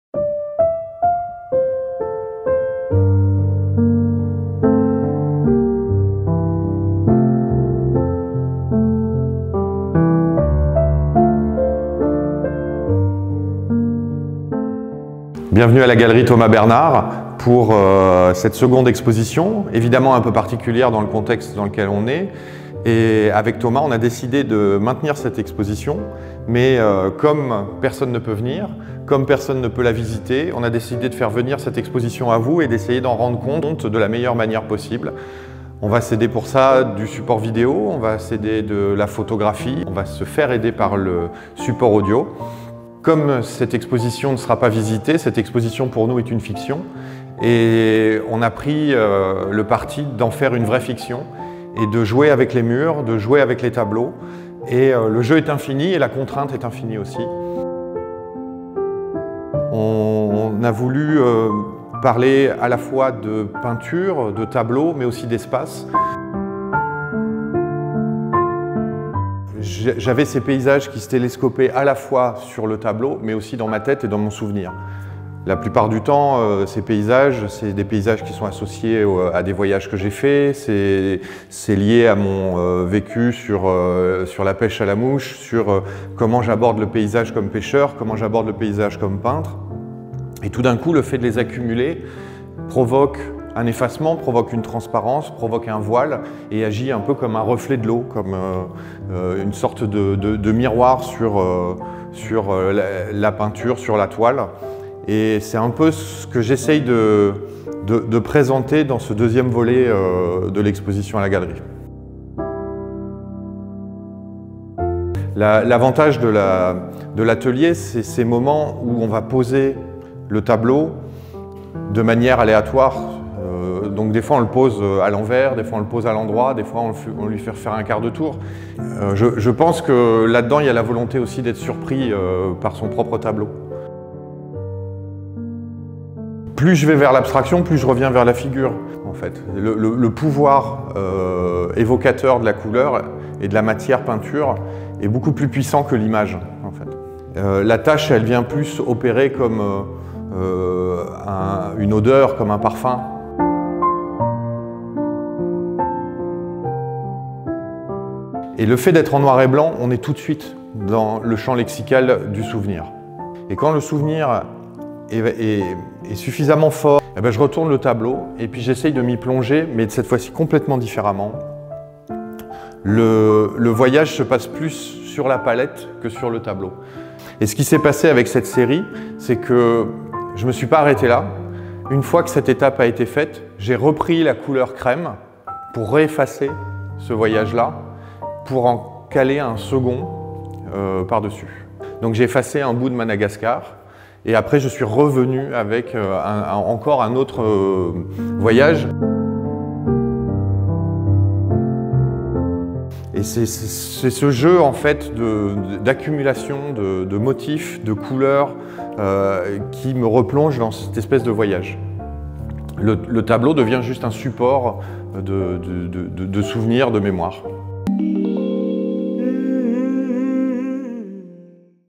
"Le Voile effacé", une exposition entre tangible et virtuel - Olivier Masmonteil
Très heureux de vous présenter mon exposition "Le Voile effacé" à la Galerie Thomas Bernard - Cortex Athletico... mais sous une forme toute particulière dans le contexte actuel !

Nous faisons le pari de donner vie à cette exposition, d'en créer une expérience sensible - grâce à des vidéos, photos, podcasts - mais surtout grâce à vous et à votre partage !

Avec cette vidéo, je vous invite à vous balader virtuellement à travers les oeuvres accrochées aux murs de la Galerie Thomas Bernard à Paris, et je compte sur vous pour relayer l'évènement et me poser vos questions directement en commentaire de ce post.

Très belle visite à tous,